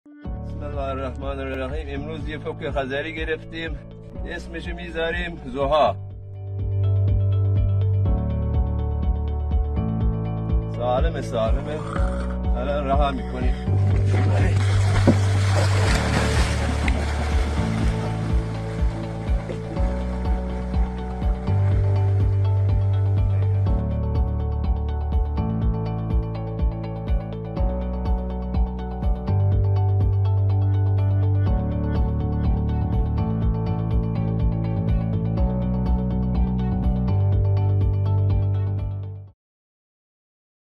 آزادسازی یک فک کاسپین در آب‌های مرزی ایران و ترکمنستان
«مرکز حفاظت فوک کاسپین ایران» ویدیویی منتشر کرده است که آن در گروهی از ماهیگیران، یک فوک ماده کاسپین را پس از به دام افتادن در تورهای صیادی در آب‌های مرزی با کشور ترکمنستان، آزاد کردند.
در توضیحاتی که این رسانه روز پنجشنبه، ۱۳ مهر ماه منتشر کرده است که فک کاسپین رهاسازی شده، سالم و سرحال بوده است.